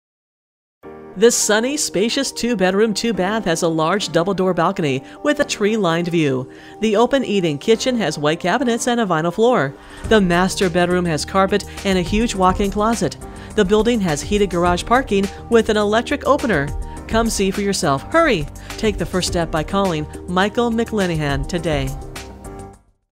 6755 N. Milwaukee Avenue Unit 411 Niles, IL | ColdwellBankerHomes.com
6755 N. Milwaukee Avenue Unit 411, Niles, IL

Sunny, spacious two bedroom, two bath(elevator building) has a large double door balcony with a tree-lined view, in-unit washer/dryer, professionally cleaned carpet 22, and natural oak woodwork throughout. The open eat-in kitchen has white cabinets, gently used appliances, and a vinyl floor. The ha